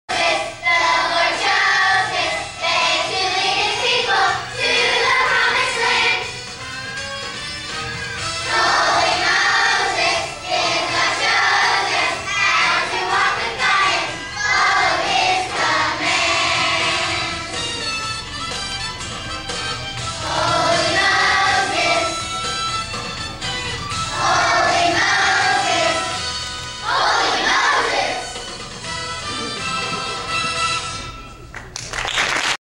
Team kids last perf 2010 (3)
Taylor Singing with Team Kids!! First Baptist Church in Goldthwaite, TX 2010 - their year end program!!